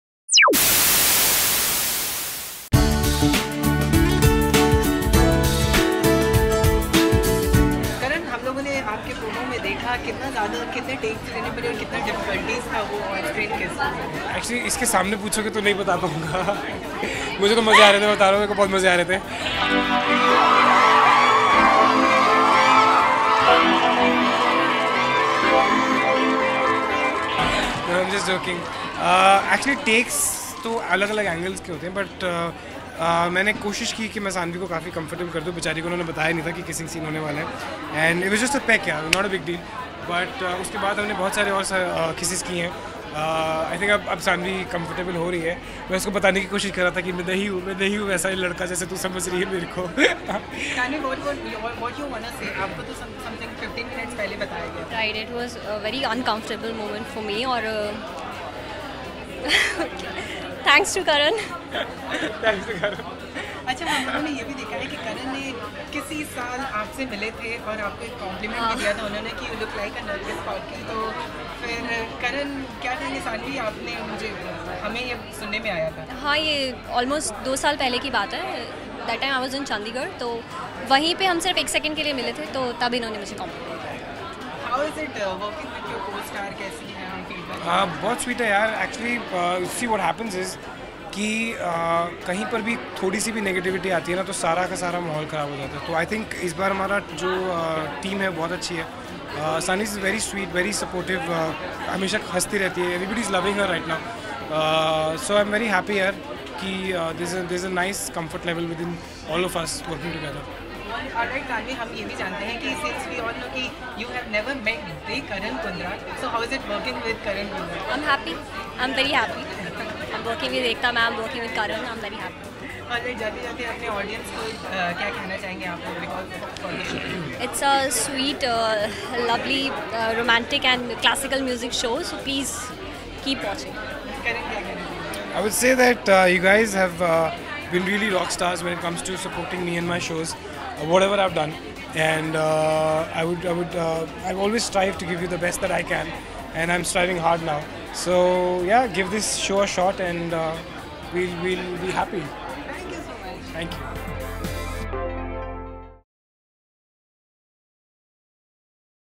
Karan Kundra aka Rahul Took Many TAKES For a Kissing Scene | Yeh Kaha Aa Gaye Hum
Handsome hunk, Karan Kundra is all excited for his upcoming daily show Yeh kaha Aa Gaye Hum on And TV. Watch the video to know what he wanna say about his on screen kissing scene.